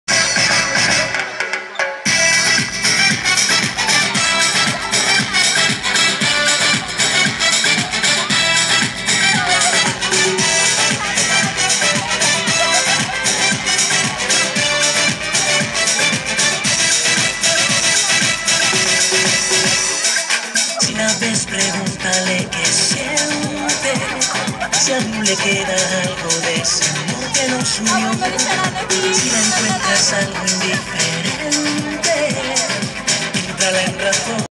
Actuació de Carnaval de Llucmaçanes 2010
Espectacle d`en David Civera a LLucmaçanes  reencarnat amb n`Esteban Martínez.